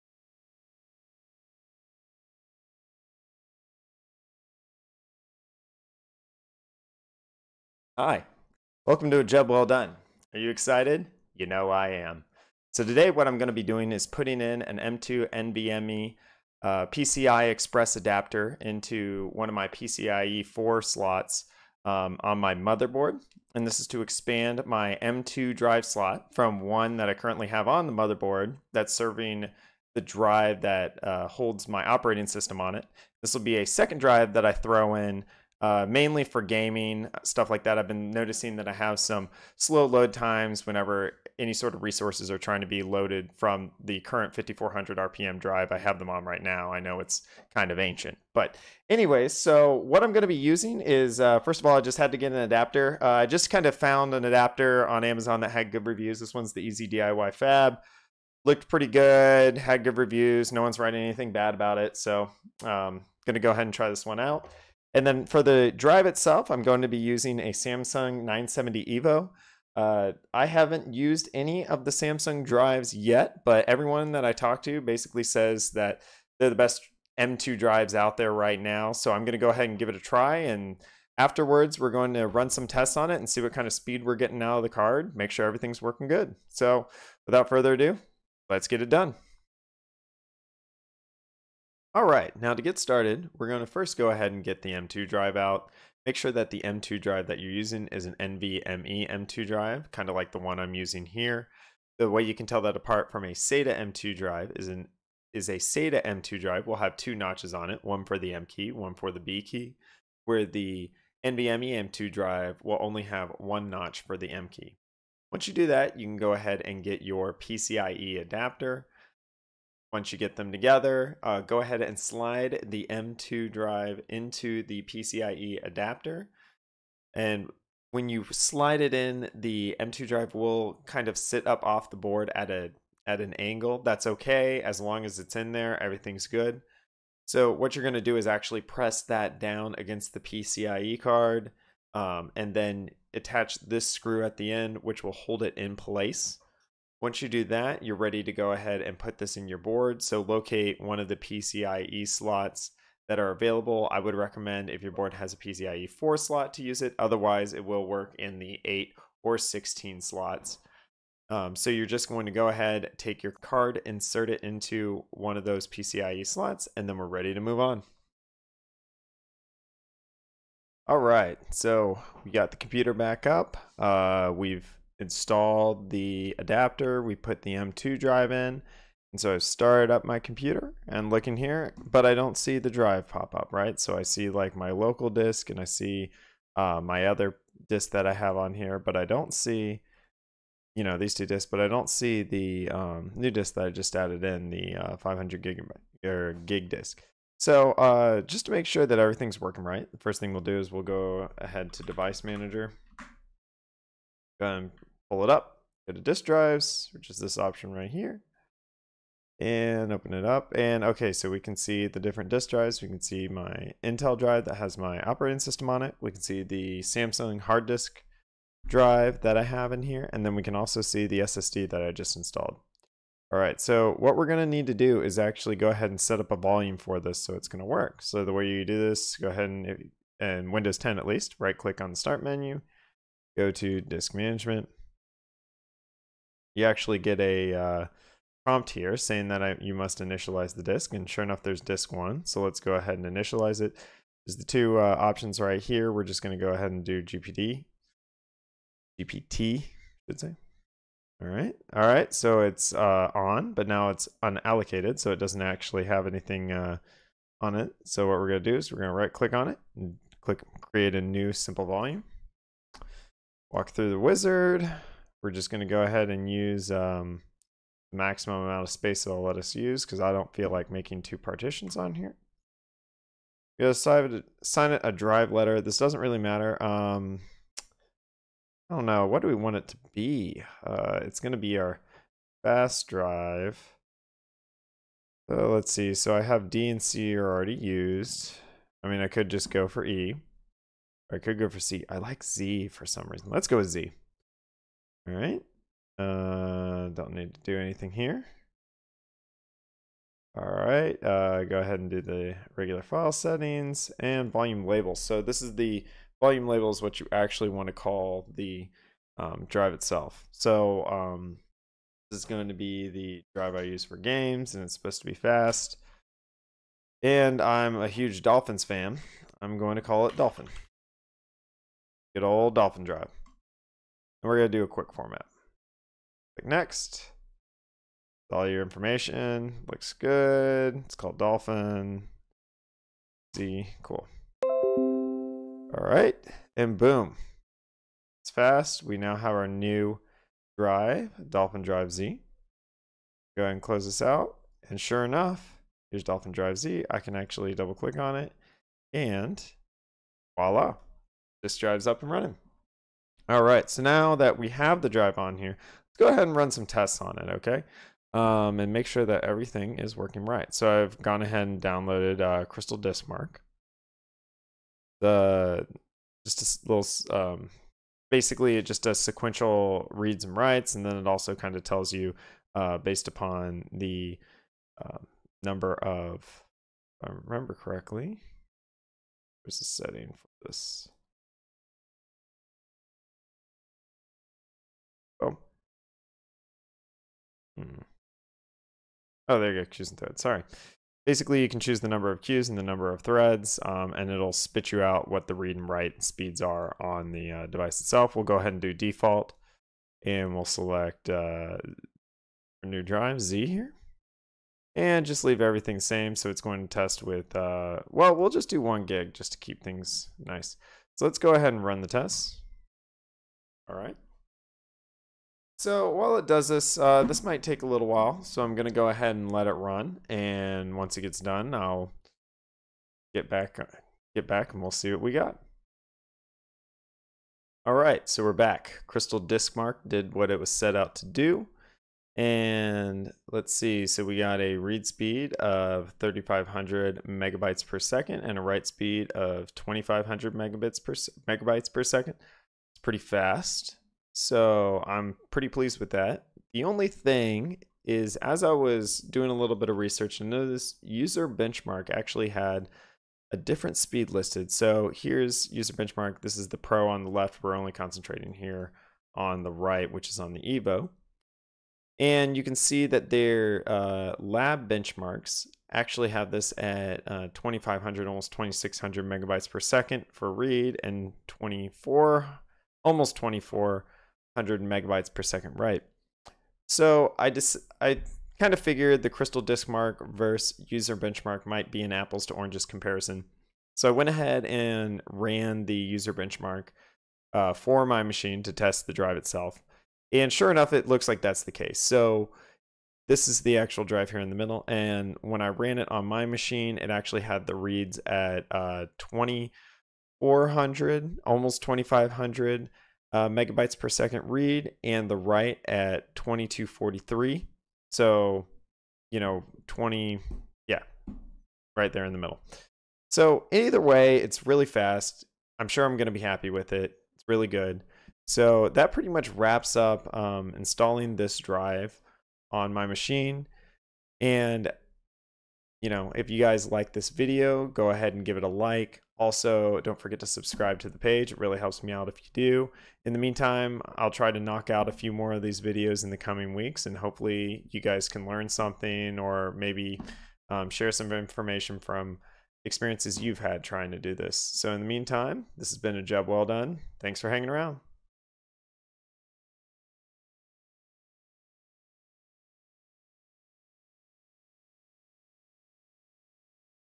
Hardware Thyme! - Installing M.2 Drive with PCIe Adapter
Video of how to install an M.2 drive into your computer using a PCIe card adapter. I run through how to install the card, initialize the drive, create a volume on the drive, and testing your drive speed.

Parts:
- EZDIY-FAB PCIe M.2 Adapter
- Samsung 970 EVO 500GB